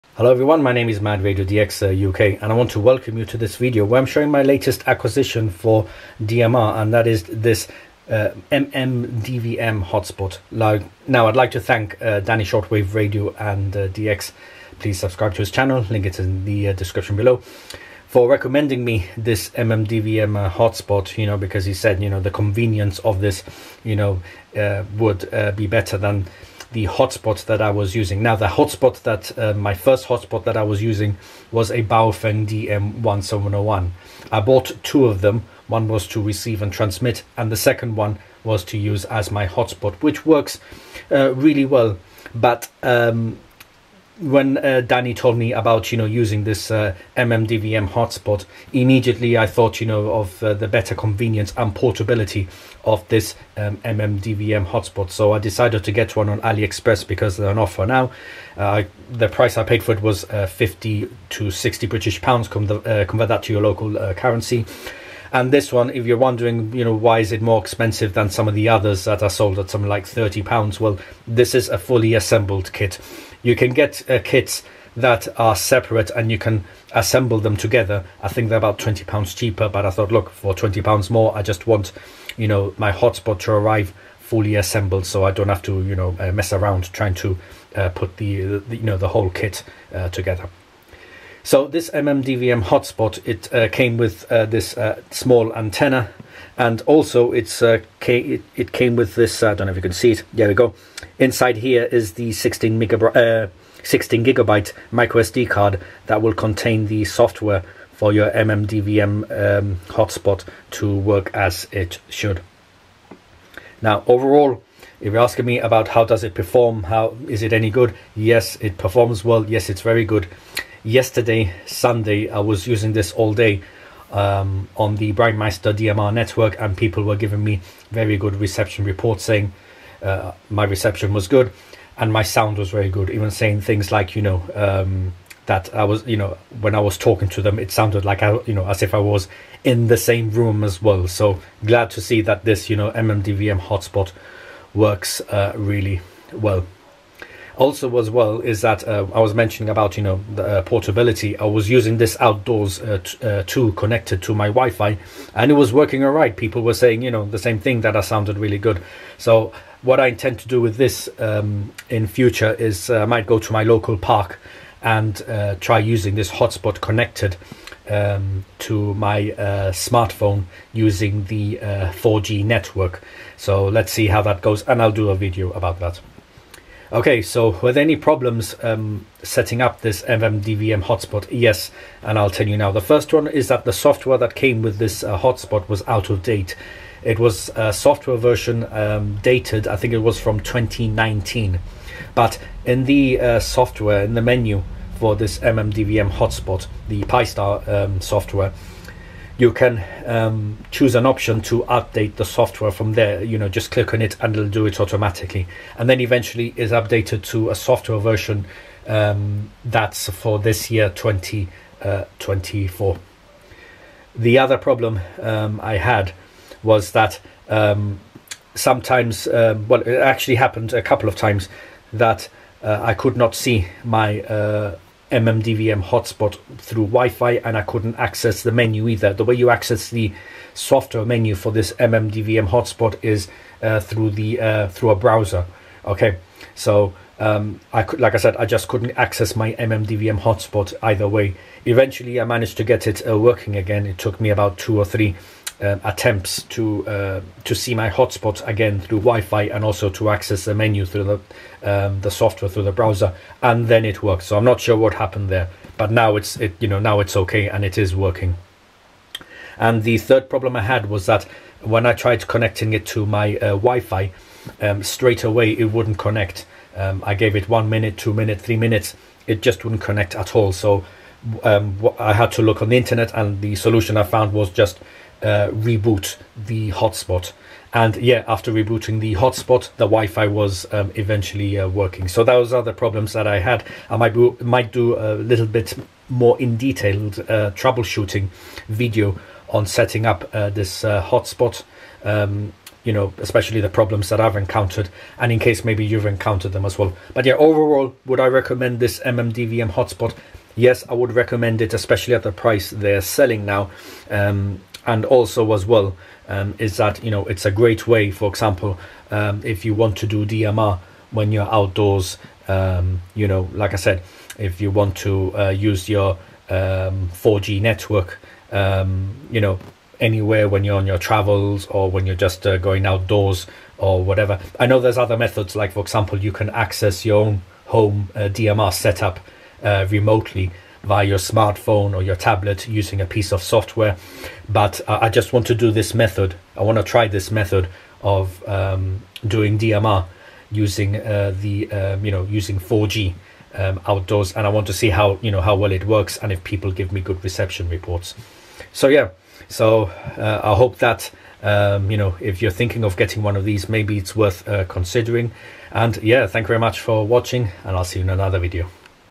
My new DMR Hotspot: MMDVM with Pi-Star, first impressions & problems I encountered
Showing my new Hotspot for DMR, which is a MMDVM with Pi-Star. I also give my initial impressions, & also the problems I encountered while trying to set this up.

00:00 Introduction
00:28 My previous hotspot
00:44 Better convenience than previous Hotspot
00:54 Where I bought this Hotspot & why
01:31 What it came with
01:54 Is it good?
02:28 Portable use of this Hotspot
02:57 Problems with this Hotspot
04:58 Would I recommend this Hotspot
05:49 Closing